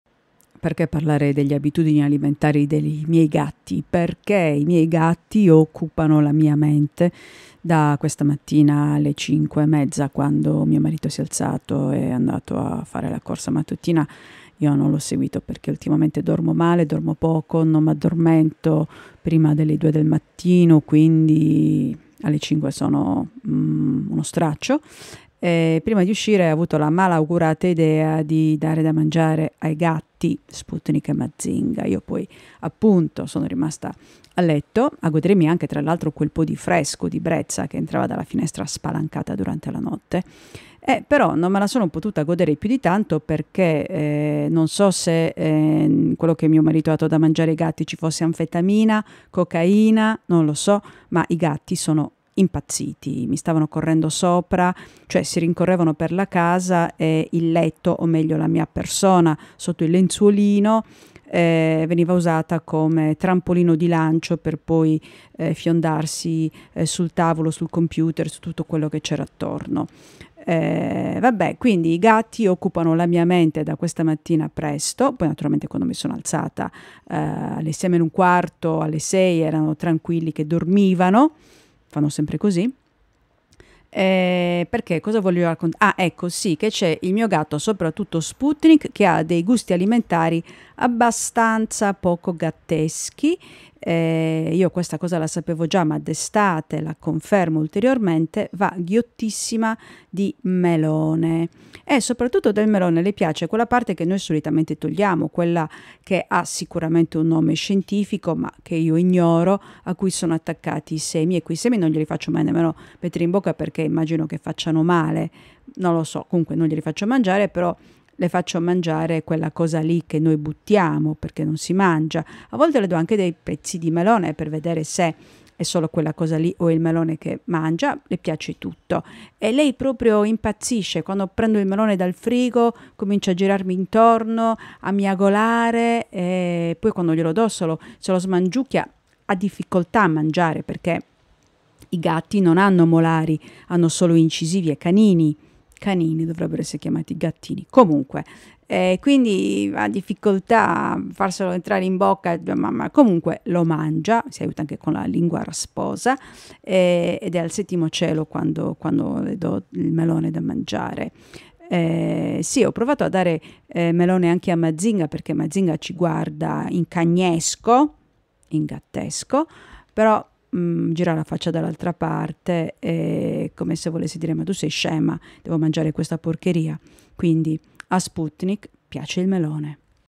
3' grezzi Sputnik's melon PODCA(S)T
I talk again about my cats who have tormented me early this morning.